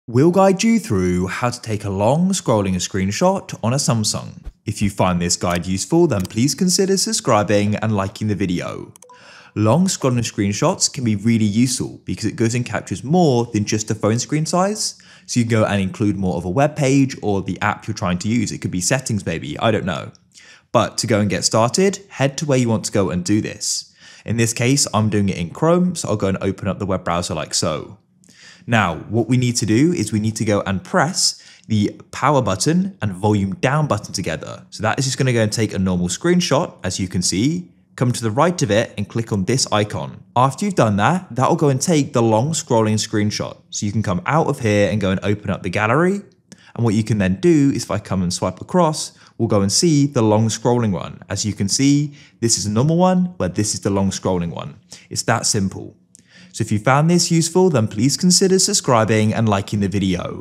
How To Take A Long Scrolling Screenshot On Samsung Phone - Full Guide
Learn how to take a long scrolling screenshot on samsung phone in this video.

GuideRealm